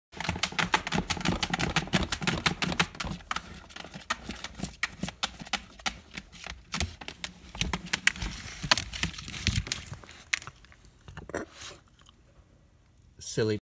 Corgi paws at carrying crate then burps.
She was upset that it was raining.